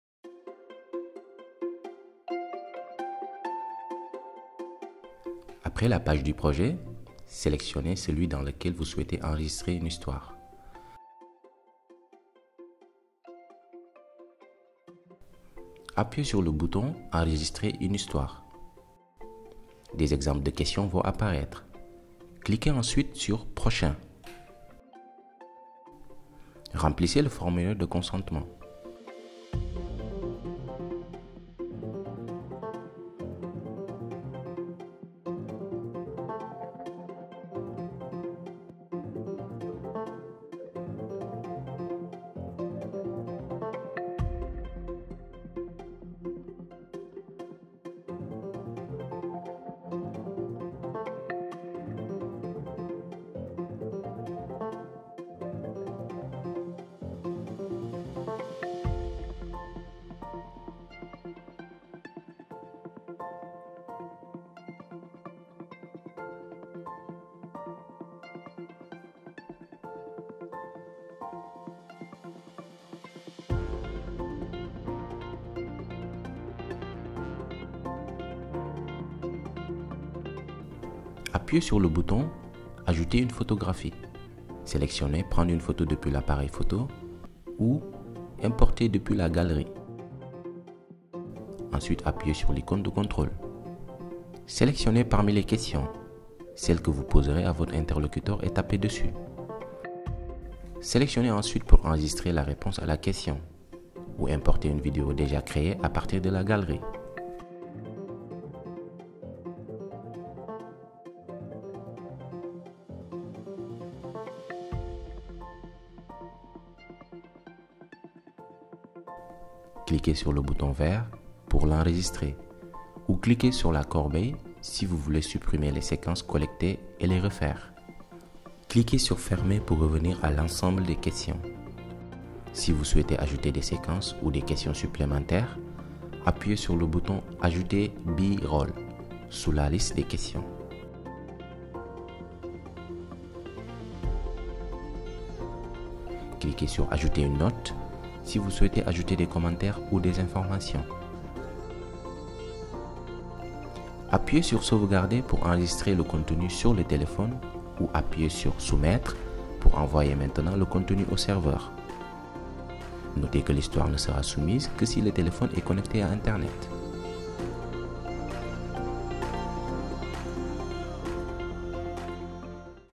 Tutoriel du CRM App #4: Recueillir des histoires
Regardez la vidéo pour découvrir comment créer des histoires en utilisant l'application CRM de l'OIM.